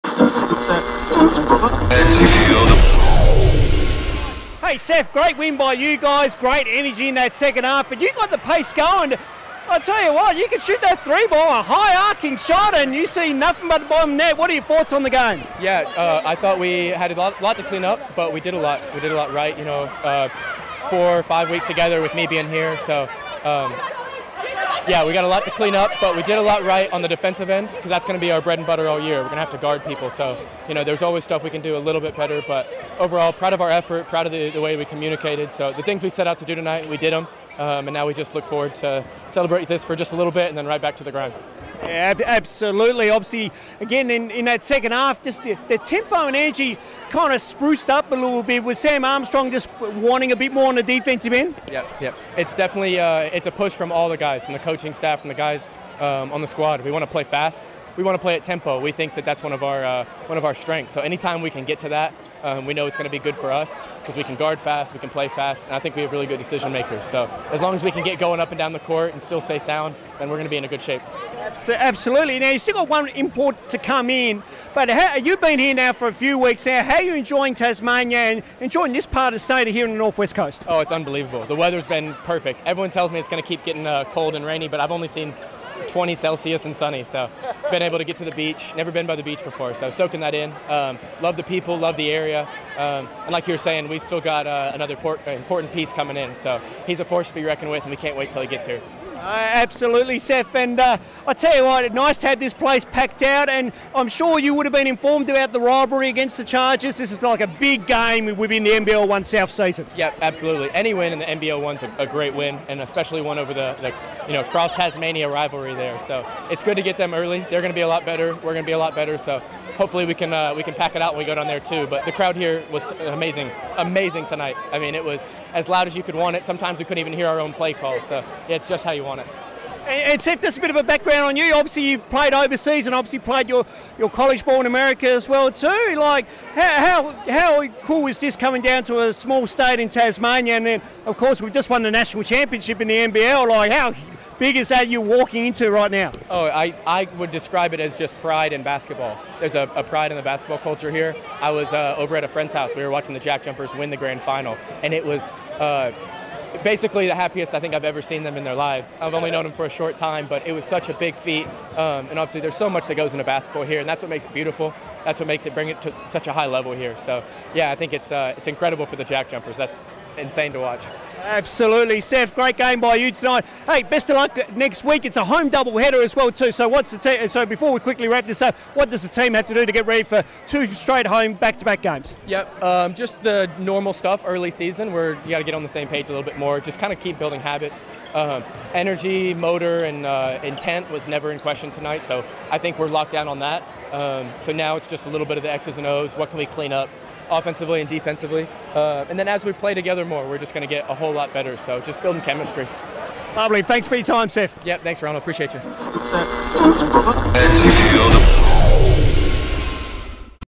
Chargers TV Post Game Wrap Round 1 Seth Swalve
Chargers TV caught up with North West Thunder player Seth Swalve to get his post match reaction as the the Thunder got the win over the Chargers by 24 points in Round 1 of the Mitsubishi Motors NBL1 South Competition.